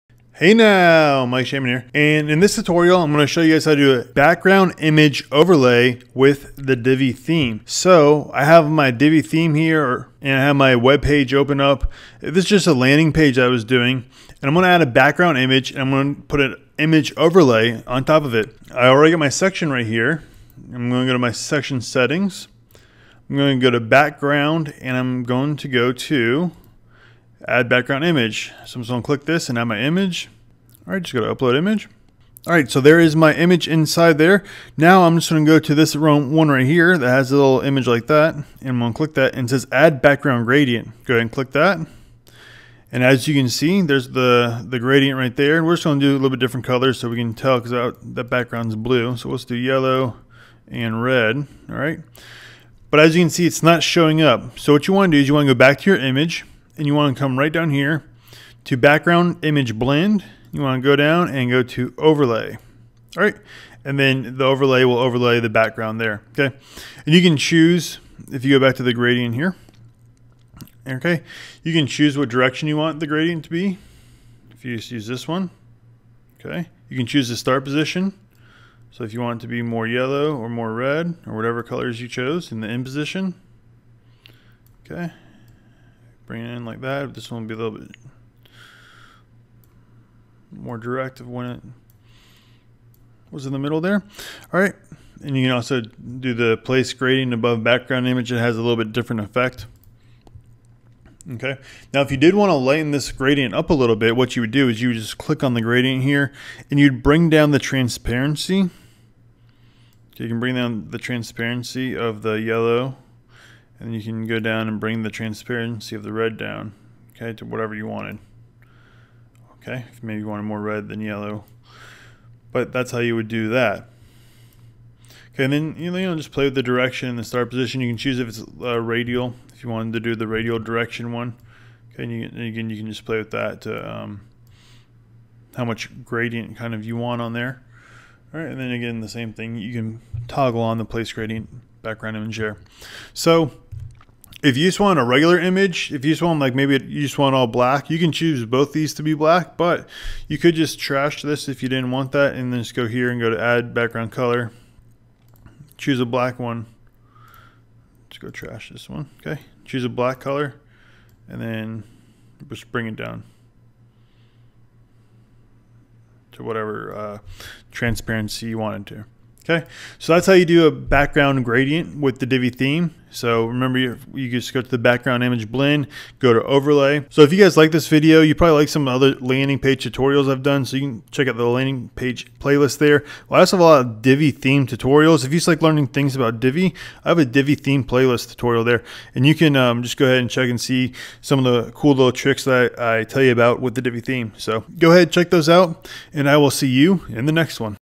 Divi Theme Background Image Gradient Overlay
Creating a gradient with background image is easy with the Divi Theme. I walk through in this tutorial how to make the gradient and the different styles you can create.

The Divi theme, from Elegant Themes, is my favorite WordPress theme because you can do things like background image gradients, toggle with transparency and add parallax effects.

One of the main reasons I like the Divi theme is the ability to import and export layouts.